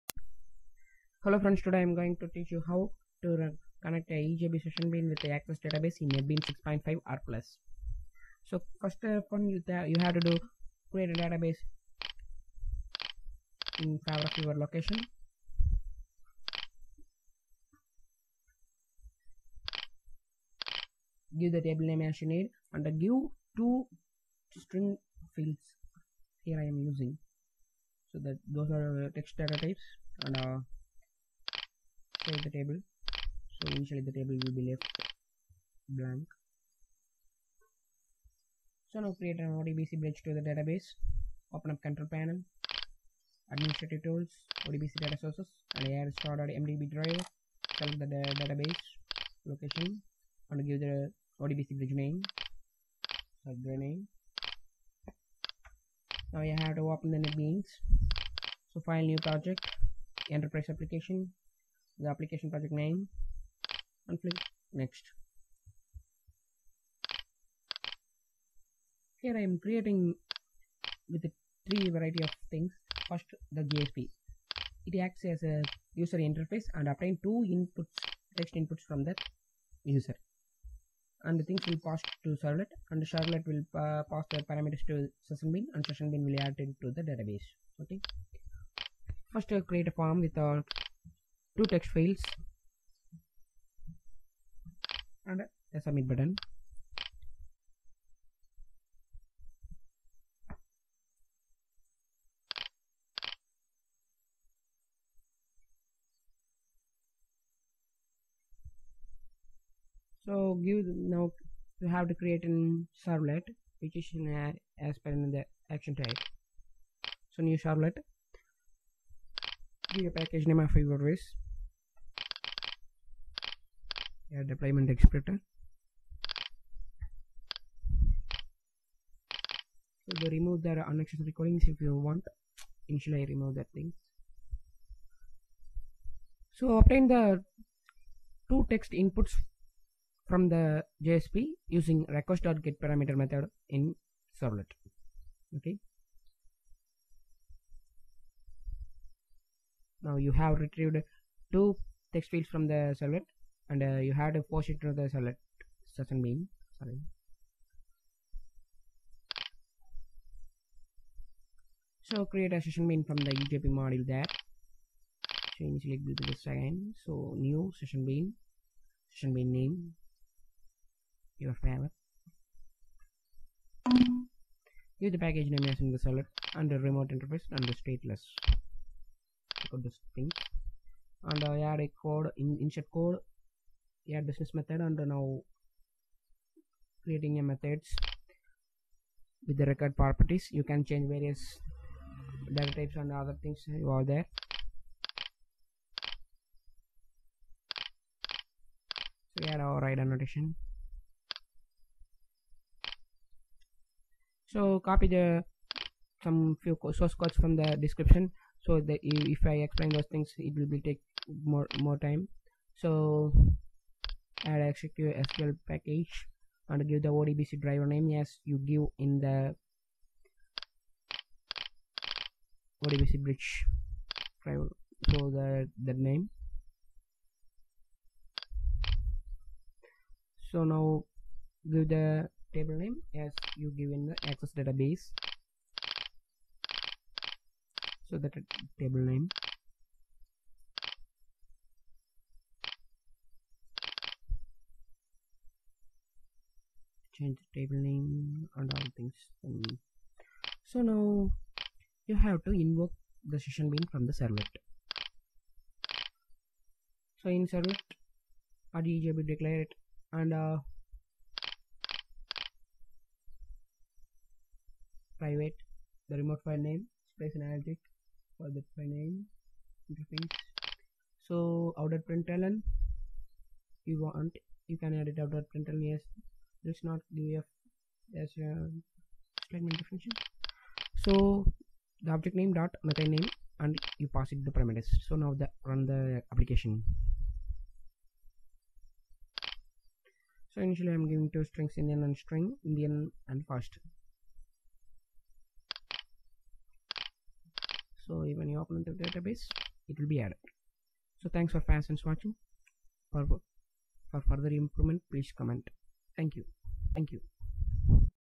EJB with MS Access In Netbeans
CODE SNIPPET ::-)
  try
       Statement st;
       Connection con=null;
       Class.forName("sun.jdbc.odbc.JdbcOdbcDriver");
       con=DriverManager.getConnection("jdbc:odbc:odbc_datasource_Name");
       st = con.createStatement();
       String sql = "insert into tablename_in_ACCESS values('"+k+"','"+s+"')";
       st.executeUpdate(sql);
       st.close();